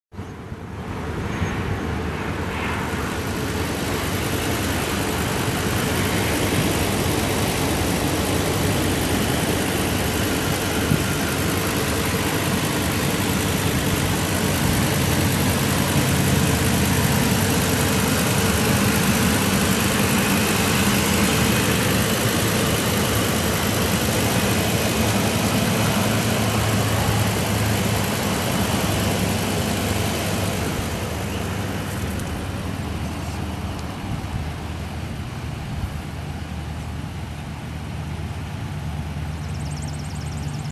2012 Ford Econoline E450 Super Duty cab and chassis at auction | bidding closes July 18, 2018
2012 Ford Econoline E450 Super Duty cab and chassis
140,740 miles on odometer
6.8L V10 SOHC 20V gas engine
Automatic transmission
Single axle
AC and heat
Tilt
Cruise
AM/FM
167"L from rear of cab to end of frame
LT225/75R16 tires
GVWR 14,500 lbs
Chipped windshield
Decals have been removed
Previously used as an ambulance
Missouri title